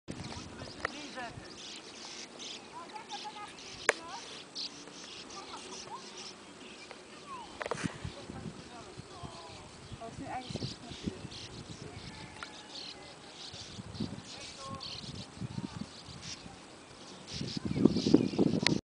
Oeverzwaluwen 1
Oeverzwaluwen aan de kiezelgroeve bij Dilsen (B)